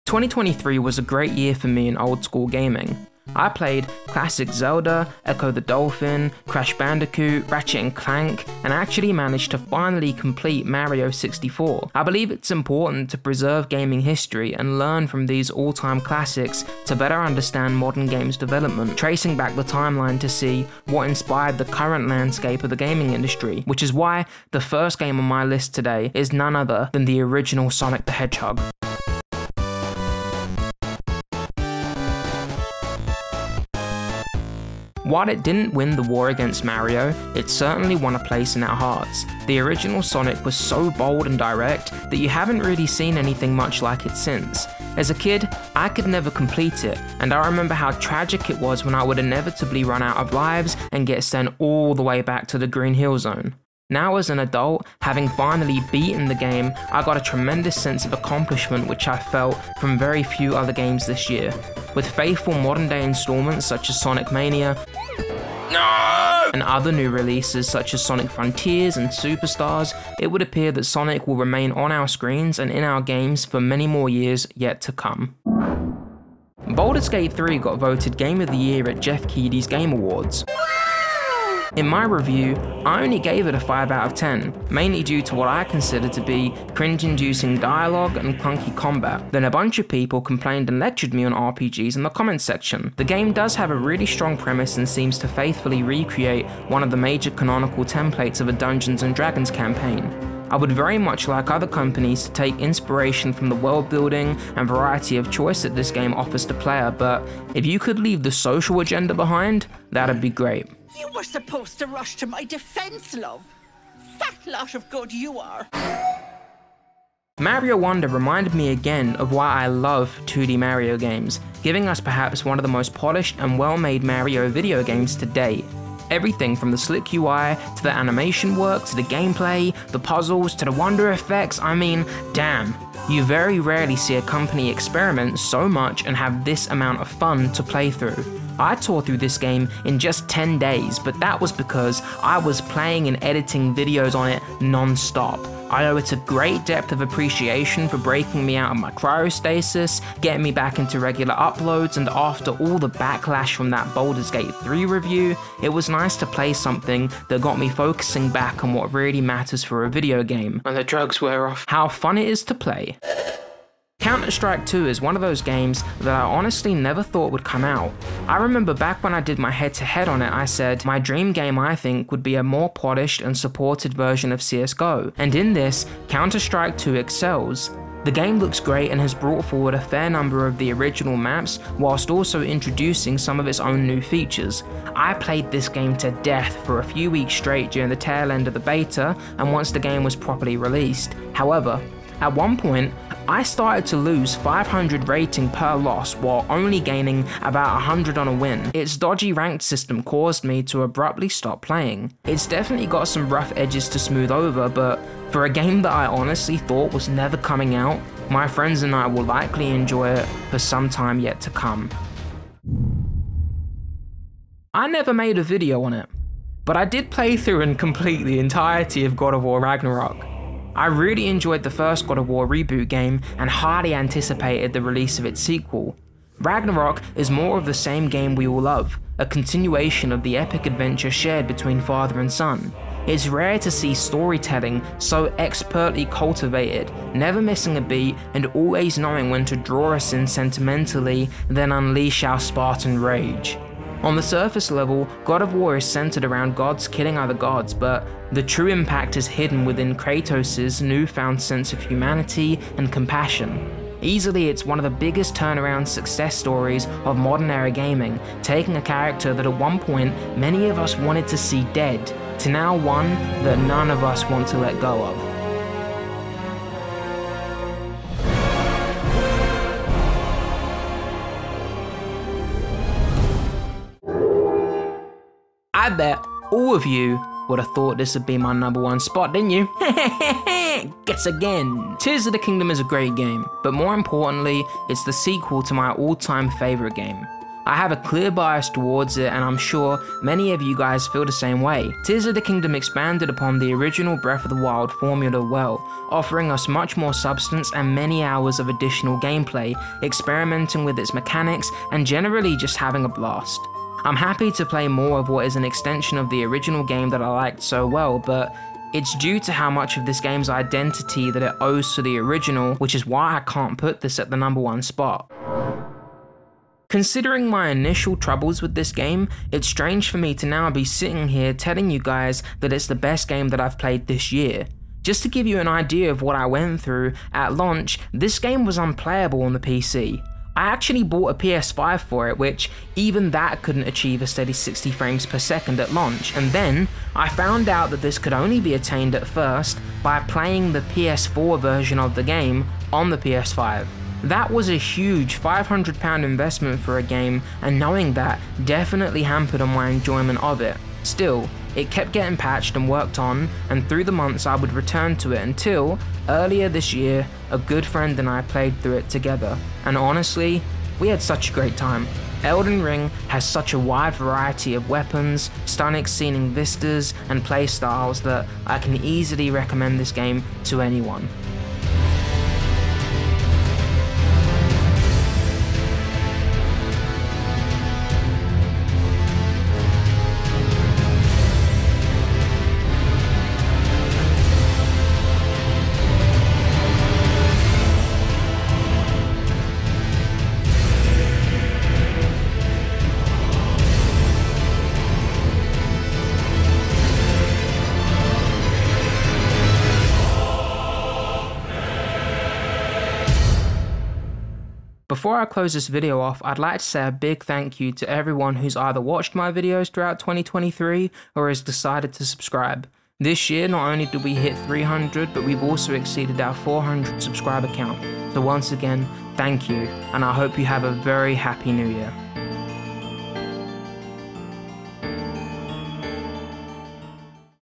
The Best Games I've Played in 2023
ABOUT
This video ranks the best games that I played in 2023
Over the past week I've playing festive, in game events and now, to finally send the year off on its way, I've ranked the games that I enjoyed playing the most in 2023.

2024 will soon be upon us. I think it's time to take a moment and appreciate some of the best games 2023 had to offer, as I'm sure they'll set the new standard for new video game releases in the gaming industry next year. Looking forward to doing this again at the end of 2024! Hopefully with just as fierce competition :D

CHAPTERS
00:00 - Intro
00:27 - 7 "Sonic The Hedgehog"
01:16 - 6 "Baldur's Gate 3"
01:56 - 5 "Super Mario Bros. Wonder"
02:43 - 4 "Counter-Strike 2"
03:33 - 3 "God of War Ragnarök"
04:34 - 2 "The Legend of Zelda: Tears of The Kingdom"
05:16 - 1 "Elden Ring"
06:45 - Outro

PRODUCTION
Edited on Adobe Premiere Pro:

PC SPECIFICATIONS

BASEBOARD - ROG STRIX B550-F
STORAGE - Samsung SSD 980 Pro (Heatsink) 1TB

THANKS

Tell me what you like and dislike aobut this movie.